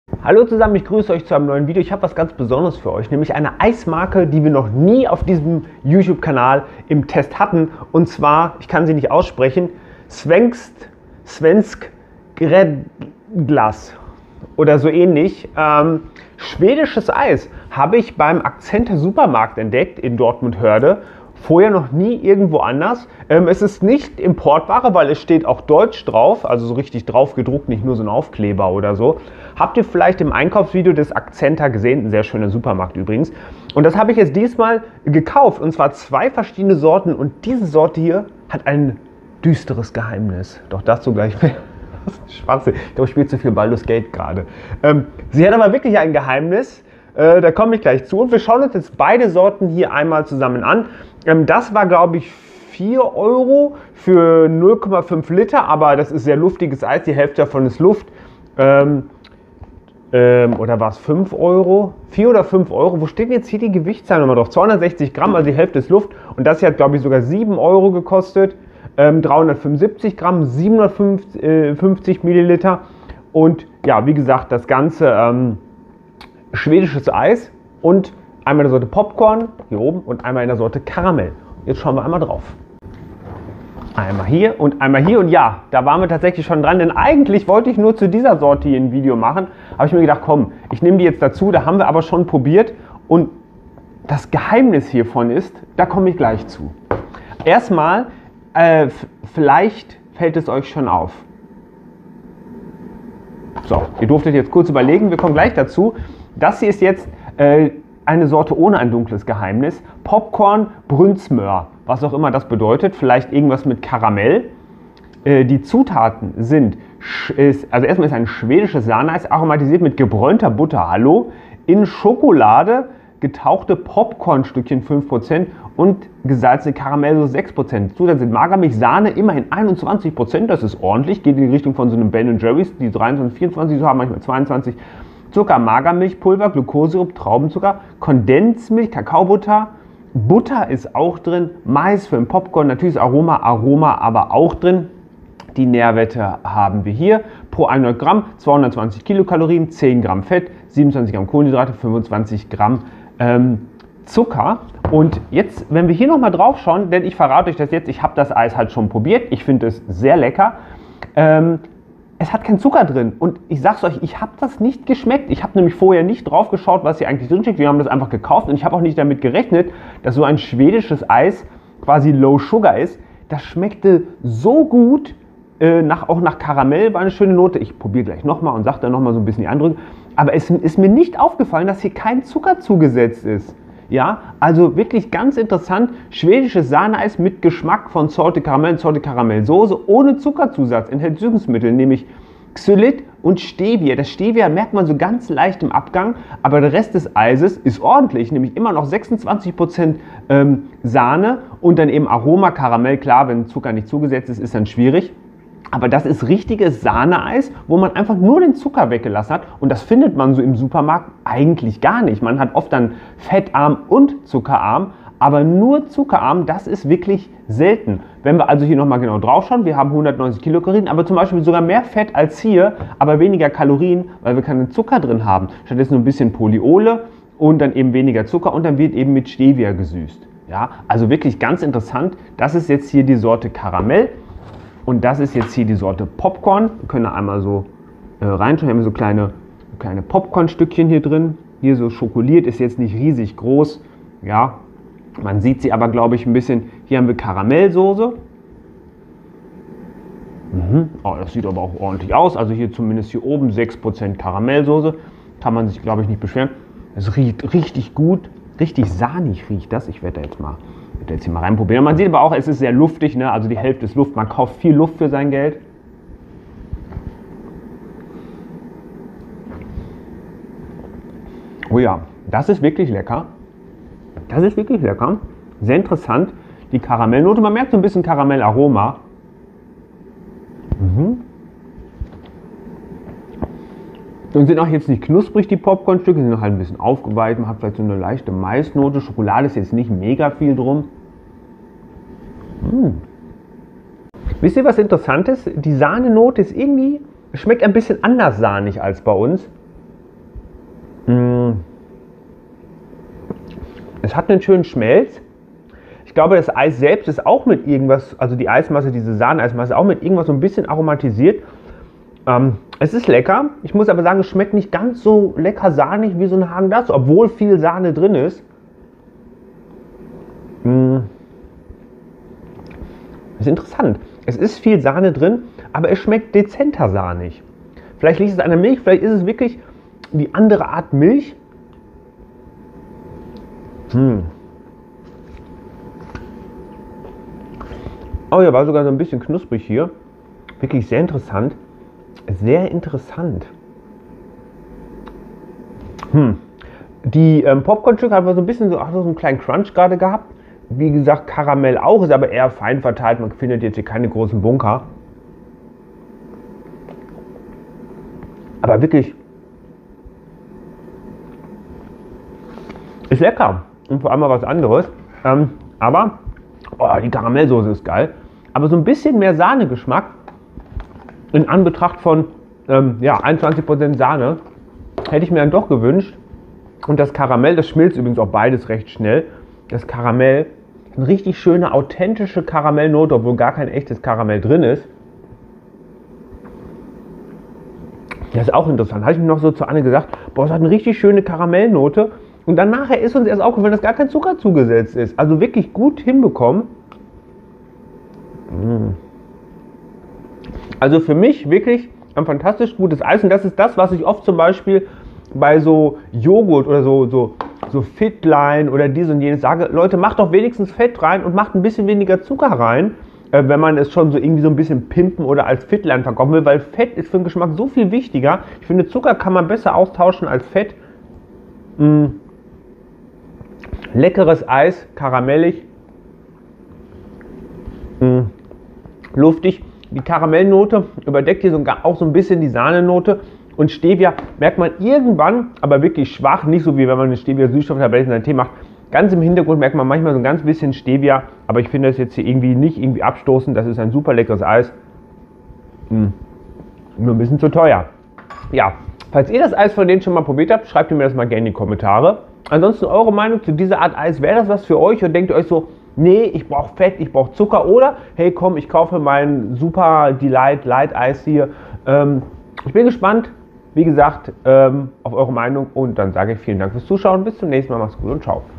Svensk gräddglass - schwedisches Eis mit Popcorn und einem Geheimnis!
Von SIA Glass Svensk gräddglass neulich Eis im Supermarkt entdeckt und ich musste zwei Becher mitnehmen. Einmal die Sorte Karamell, einmal Popcorn. Und beim Karamell gab's ne kleine Überraschung! Alle Infos zu Preis, Zutaten und Geschmack
🍒Kanalmitglied werden um mich zu unterstützen:

▬▬▬Supported mich ▬▬▬
🍒Kauft über meinen Amazon Partnerlink (1)